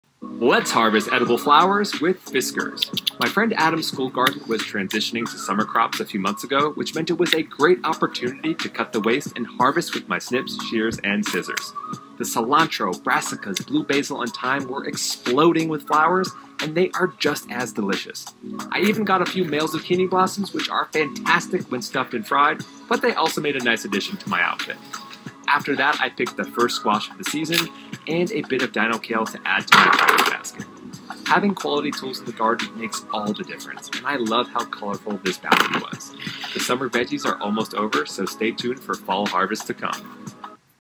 Did you know that these flowers are edible?
Before the summer started I went to my buddy Adam’s school garden to harvest the flowering herbs and veggies. Harvesting these flowering or “bolting” plants is a great way to CutTheWaste, so I grabbed my @fiskars snips, shears and scissors and got to work ✂️⁣
The herb flowers have to be my favorite because they are packed with flavor but I also really love the squash blossoms. Stuffing them with vegan cheese and pan frying them is a delicious treat, but remember to only harvest the male flowers after pollination has started since they won’t produce any zucchini 🌼⁣
So many edible plants can be harvested at different stages in their life cycle and having the right tools will help you create an abundant harvest basket every time 🥬🥒🍆🌻